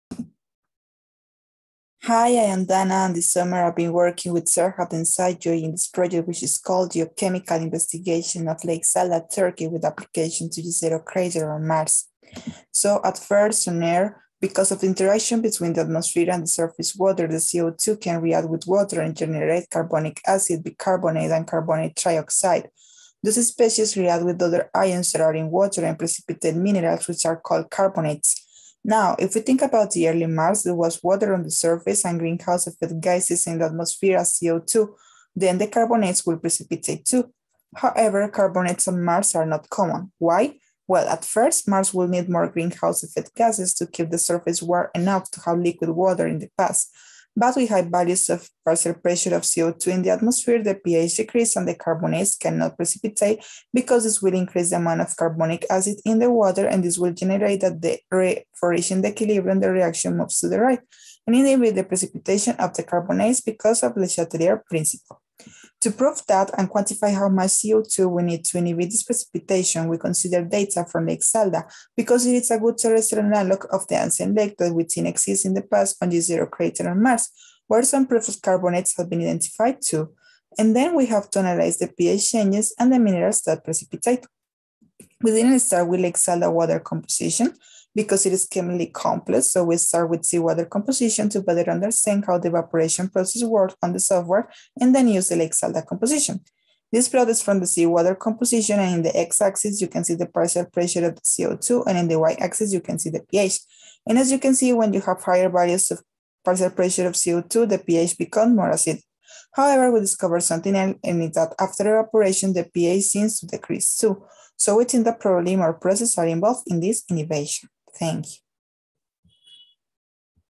Danna Jaimes Fajardo - Geochemical investigation of Lake Salda, Turkey
Full title:
Geochemical investigation of Lake Salda, Turkey, with application to Jezero Crater, Mars